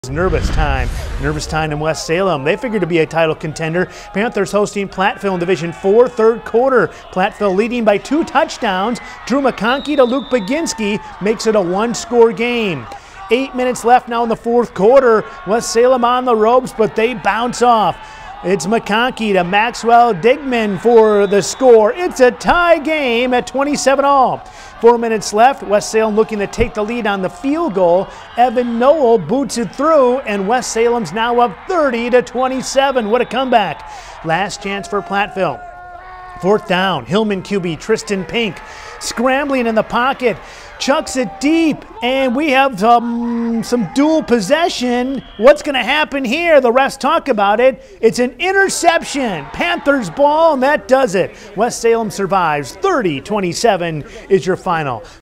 West Salem squeaks by with a win
West Salem eliminates Platteville 30-27.